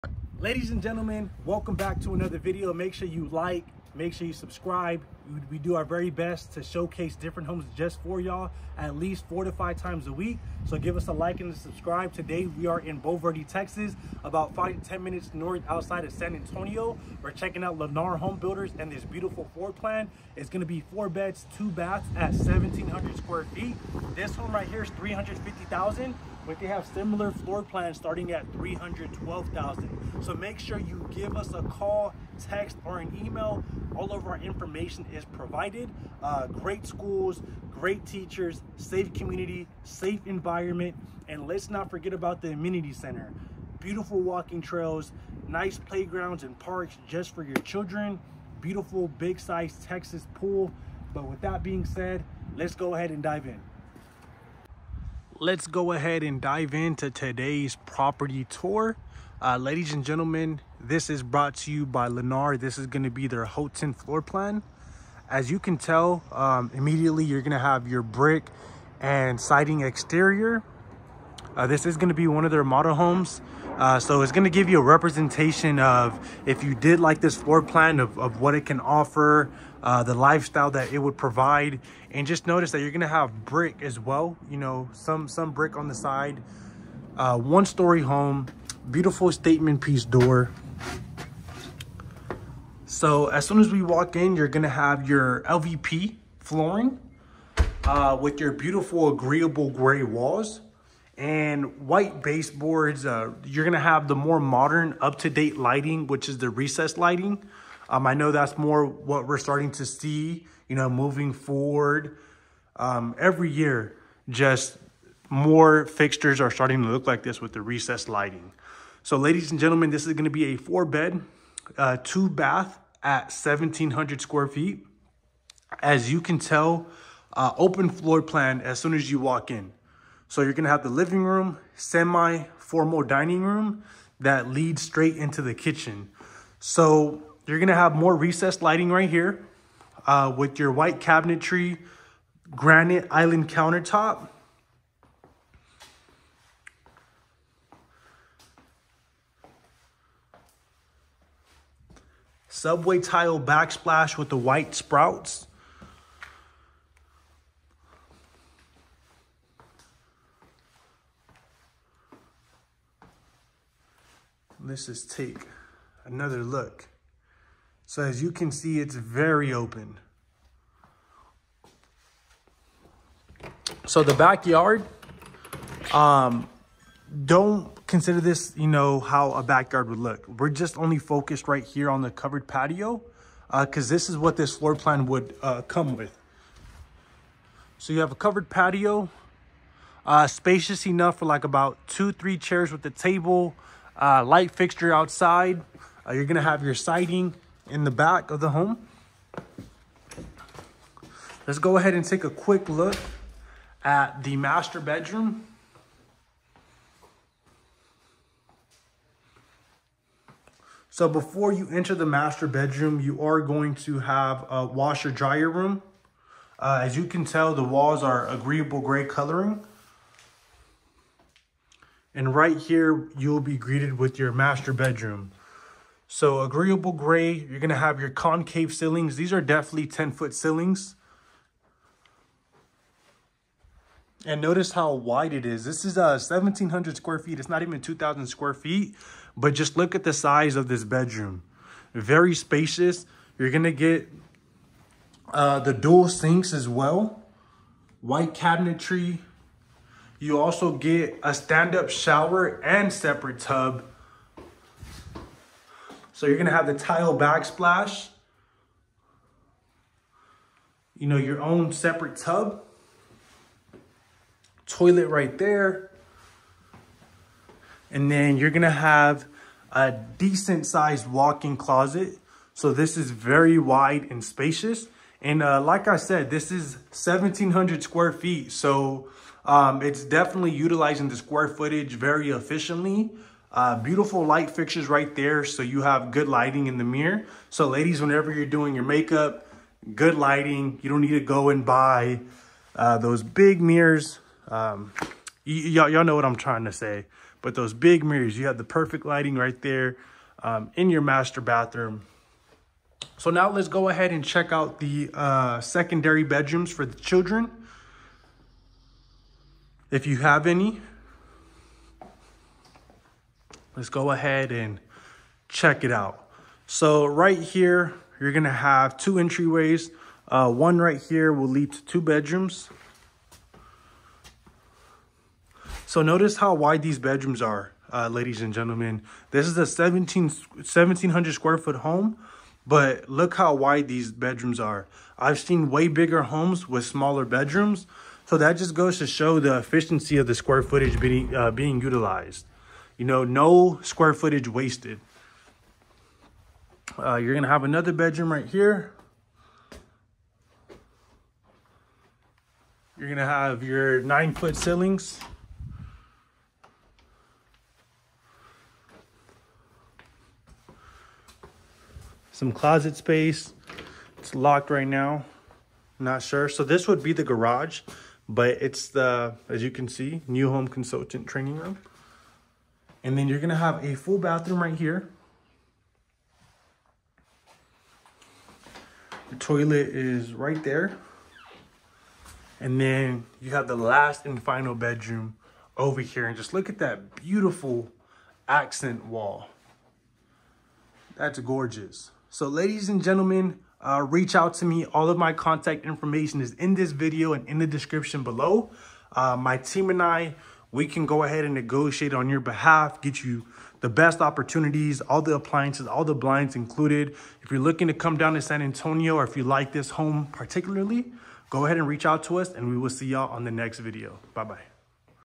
MUST SEE!! BRAND NEW CONSTRUCTION HOMES FOR SALE IN BULVERDE, TEXAS
Brand new construction homes for sale in Bulverde, Texas right near San Antonio, Texas

Time Stamps
0:00 - Amenity Center
0:57 - Exterior of home
1:41 - Living Room
2:19 - Semi dining room
2:23 - Kitchen
2:58 - Covered Patio
3:32 - Wash room
3:46 - Master bedroom
4:06 - Master bathroom
5:25 - Secondary bedrooms